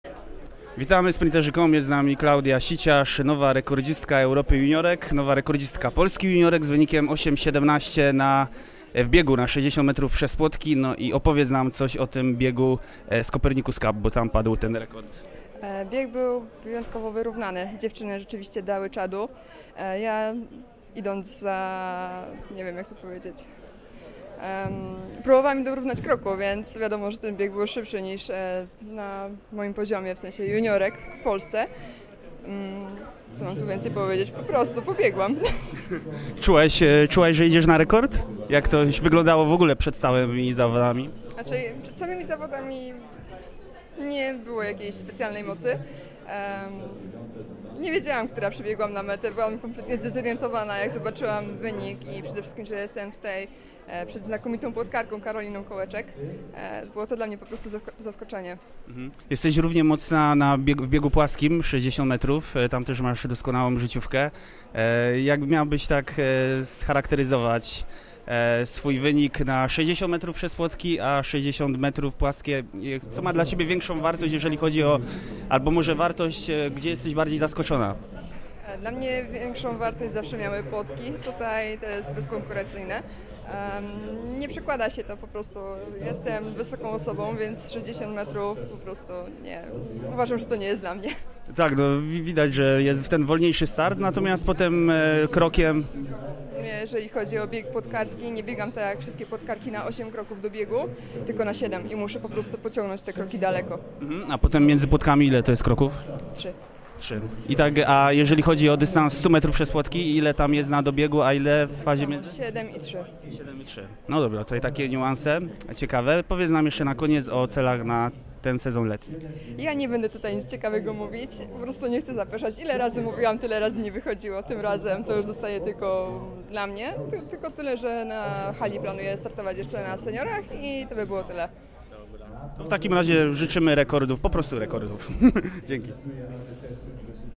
Klaudia Siciarz: "Było to dla mnie zaskoczenie"
Klaudia Siciarz, nowa rekordzistka Europy juniorek w biegu na 60M ppł. z czasem 8.17 sekundy.